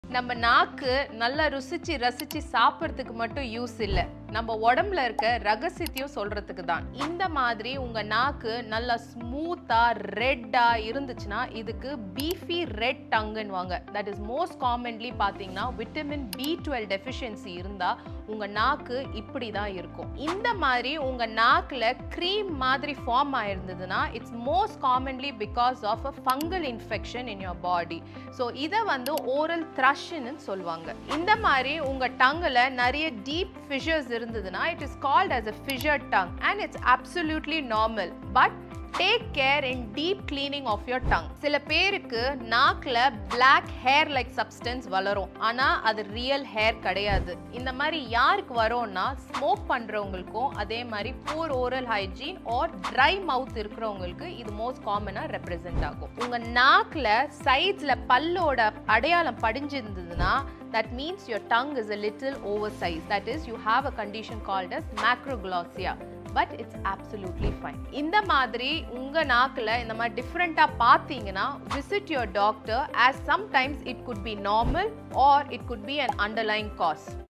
Check Your Tongue | Dr. Preethi Nadagopal
Types of tongues and their anomalies, along with treatment plans:

1.Vitamin B12 Deficiency

🔴 "Smooth & Red – The Burning Tongue!"
A deficiency in Vitamin B12 can cause a swollen, beefy red tongue with a smooth appearance and a burning sensation. You may also experience numbness and tingling in your mouth.

💊 Treatment: Increase Vitamin B12 intake through supplements, dairy, eggs, fish, and fortified cereals. If severe, your doctor may prescribe B12 injections.

2. Oral Thrush (Fungal Infection)

⚪ "White Patches "
Oral thrush appears as thick, white, cottage cheese-like patches on the tongue and inner cheeks. It is caused by an overgrowth of the Candida fungus, often due to a weakened immune system, diabetes, or prolonged antibiotic use.

🩺 Treatment: Antifungal medications (oral gels or tablets), good oral hygiene, and reducing sugar intake. If dentures are used, ensure they are cleaned properly.

3. Fissured Tongue

🌀 "Cracked but Not Broken!"
Deep grooves or cracks on the tongue’s surface—often painless but can trap food and cause irritation. This condition is usually harmless but linked to conditions like vitamin deficiencies.

🛠 Treatment: No cure is needed, but regular tongue cleaning with a soft brush and staying hydrated can prevent discomfort and bad breath.

5. Macroglossia (Enlarged Tongue)

👅 "The Oversized Tongue Struggle!"
A large tongue may cause difficulty in speaking, chewing, and even breathing. It can be due to genetic conditions (like Down syndrome), hypothyroidism

🏥 Treatment: Treating the underlying condition helps. In severe cases, surgery may be required. Speech therapy might also help improve speech and swallowing issues.

6. Black Hairy Tongue

🖤 "Looks Hairy But It’s Just Overgrown Papillae!"
A black or brown furry appearance on the tongue, caused by an overgrowth of papillae that trap bacteria, food, and stains (especially from smoking, coffee, or poor oral hygiene).

🦷 Treatment: clean your tongue daily, improve oral hygiene, quit smoking, and reduce coffee/tea intake.

📍Apollo Dental Clinic, Ambattur, Chennai.